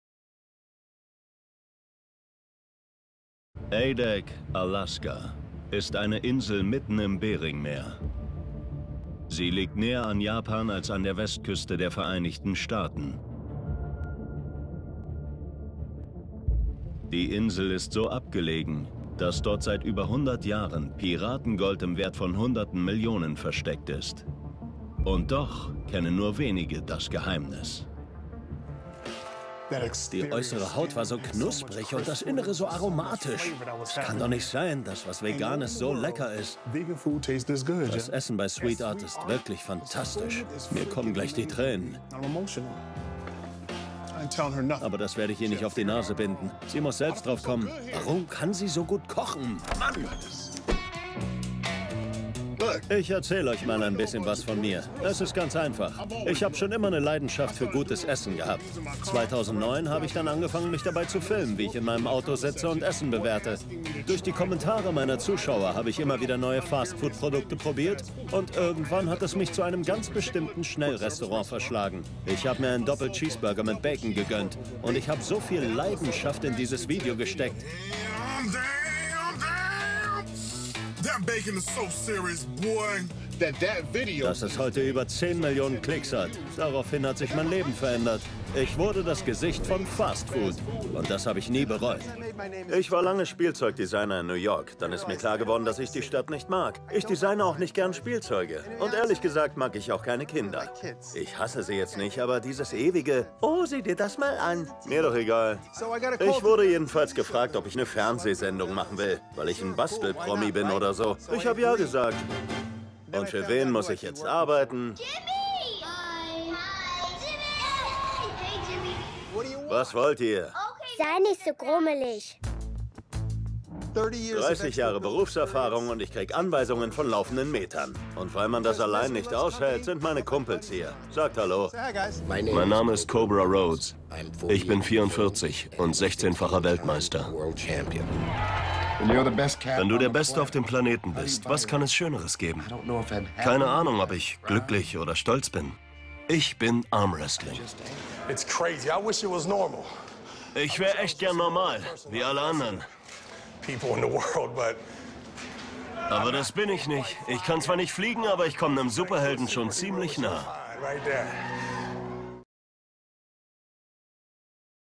Kevin Kraus (Björn Landberg) Doku/Voice-Over
Zusammenschnitt einiger Doku/Voice-Over Rollen